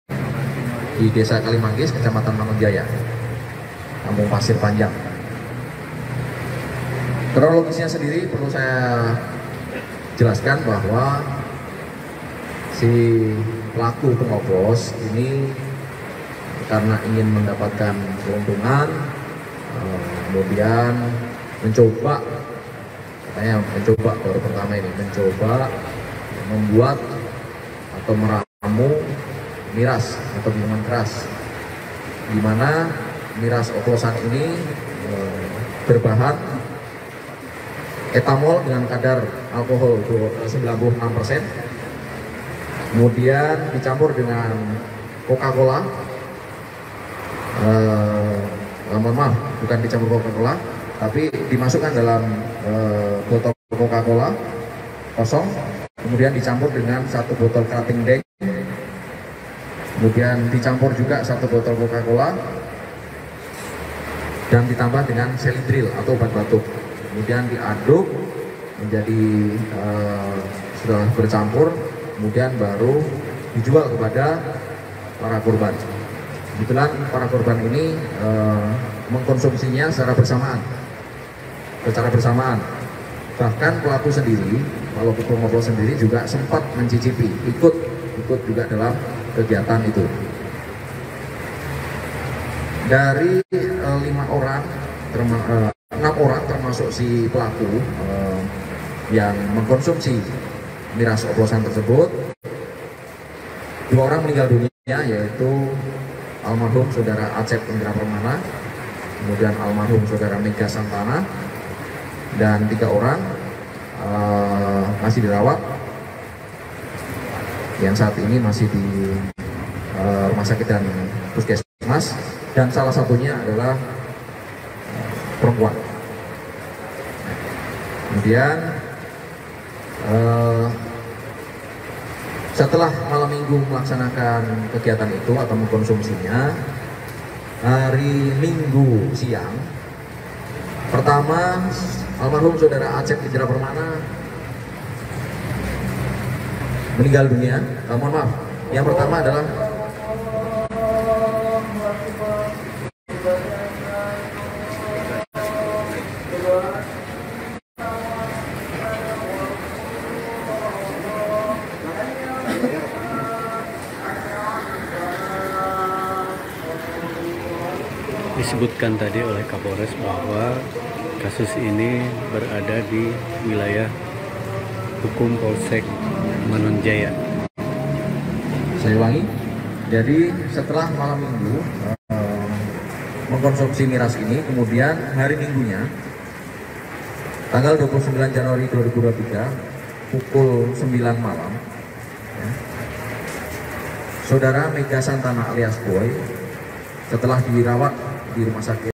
Dua Warga Tasik Tewas, Usai Tenggak Miras Racikan
Inisiatif racik Miras oplosan pakai alkohol 96 persen dua warga Manonjaya, Tasikmalaya, Jawa Barat tewas sia-sia.

Kasus tewasnya dua warga Tasikmalaya setelah menegak minuman keras (miras) oplosan berawal dari nongkrong pada malam minggu.

Kapolres Tasikmalaya Kota, AKBP Aszhari Kurniawan menceritakan kronologi pesta miras berujung maut ini.

Laporan naskah: Desi Selviani
Editor Video: Miftahul Munir